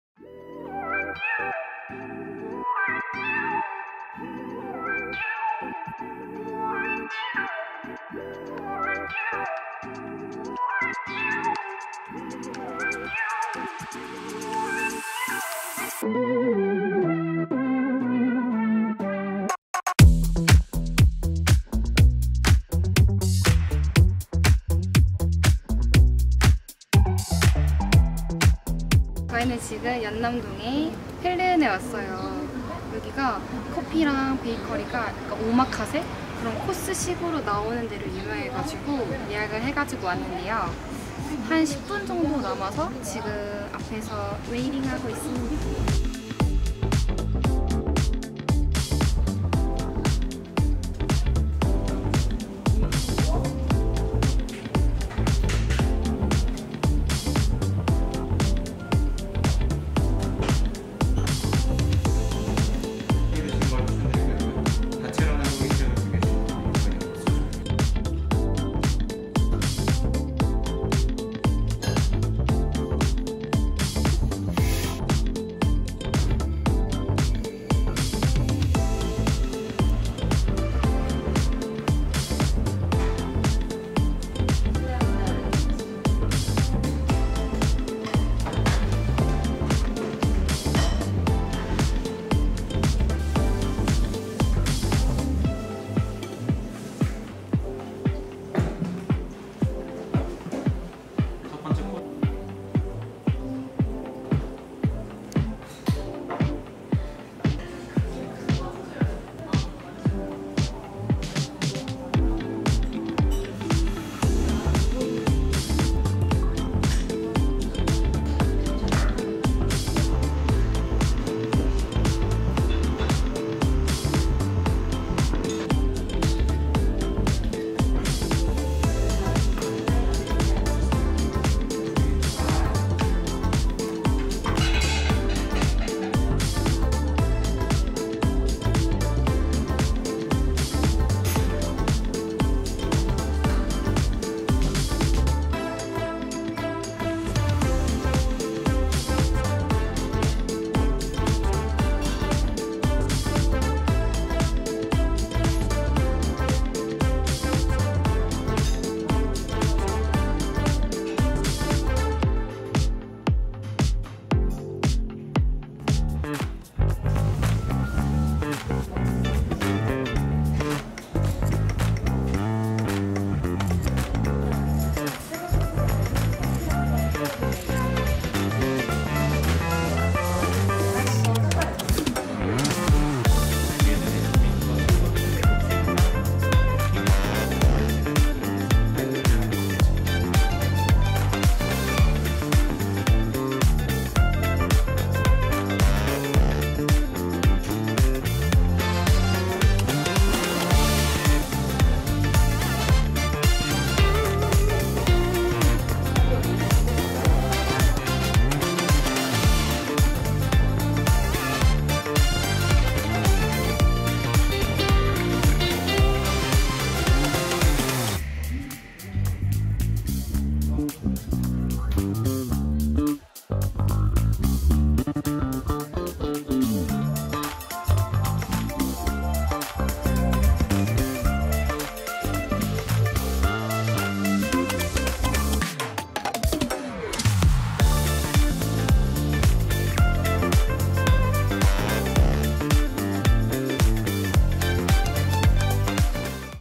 연남동 펠른 후기🥤⎮커피 오마카세⎮식스센스에 나온 펠른 다녀왔어요!
순이에요 :)

오늘은, 커피 오마카세로 잘 알려진
연남동의 펠른을 다녀왔답니다!

식스센스에도 나온 곳이라
이미 많이 유명한데요!
펠른은 어떤지 영상으로 함께 확인해보시죠 :)